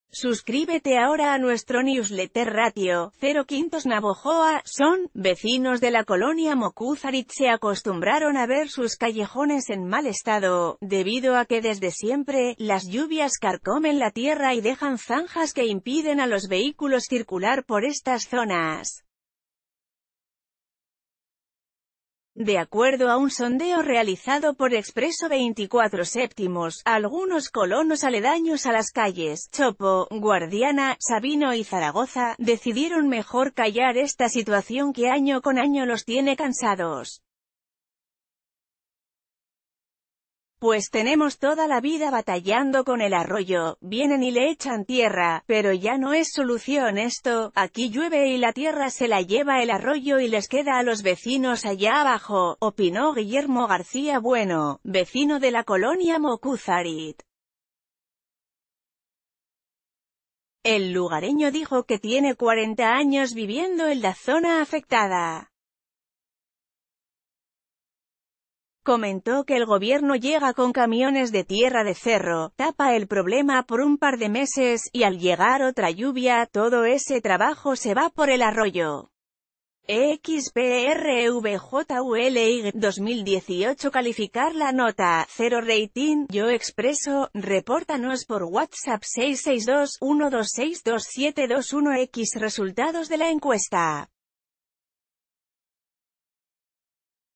AUDIO | Vecinos de la colonia Mocúzarit se quejan del mal estado de las calles
Suscríbete ahora a nuestro newsletter Ratio: 0 / 5 NAVOJOA, SON.- Vecinos de la colonia Mocúzarit se acostumbraron a ver sus callejones en mal estado, debido a que desde siempre, las lluvias carcomen la tierra y dejan zanjas que impiden a los vehículos circular por estas zonas. De acuerdo a un sonde...